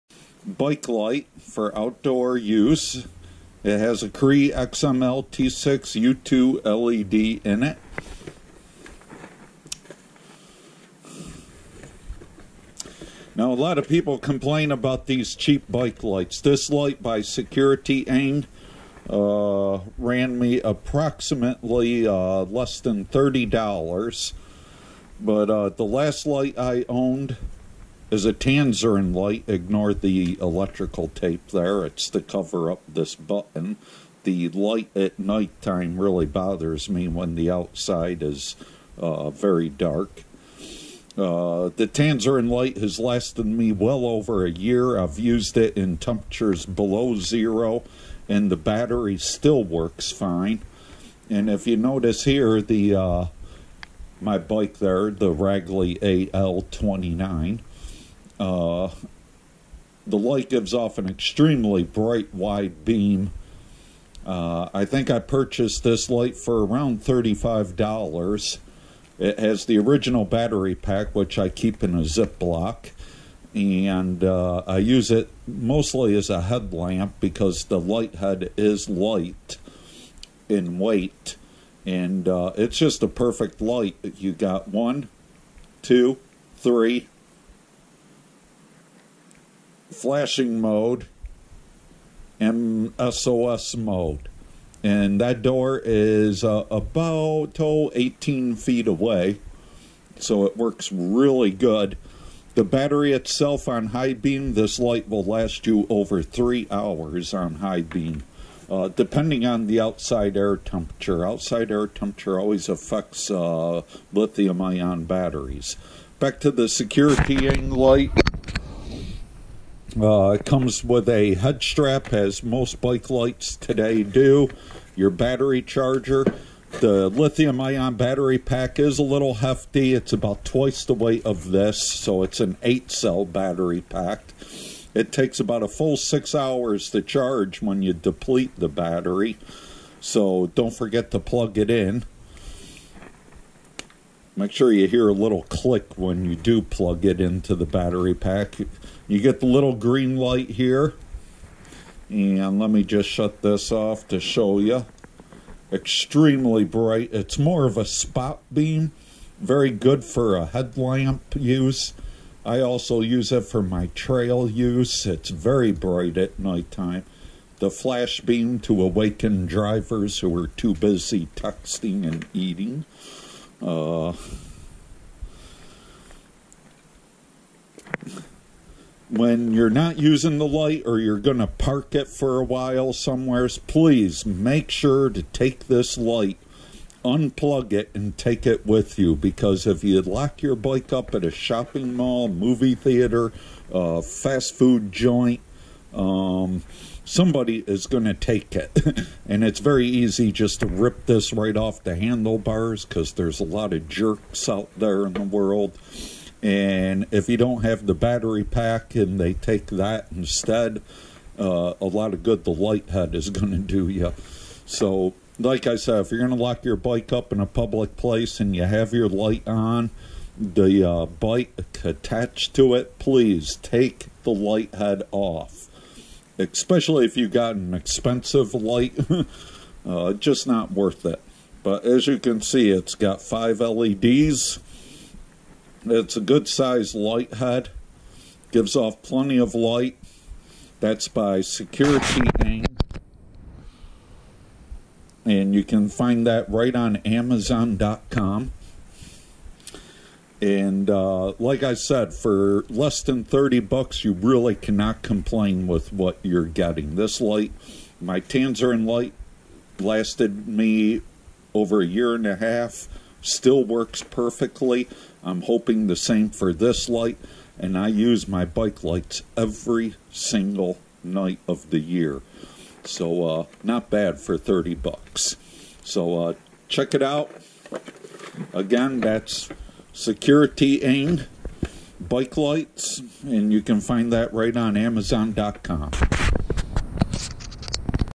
BIKE Headlight REVIEW. Securitying Light.
Securitying Lights, produce a very affordable, reliable BikeLights . Depending on outside temperatures, the battery can last over 2 hours on high beam, 4-5 hours on low, and eight hours flash mode.
For under $30.00, you can gripe about what you receive. Nor should you gripe about a bicycle light that can cost a few hundred dollars.
If it lasts one year, or even six months, it is worth-the-price. My last light was a Tansoren bike light, from Amazon. The original battery pack still works after more than one year of constant use. Not bad for a light under $40.00. I would confidently compare these AffordableBikeLights , to more expensive brands anytime.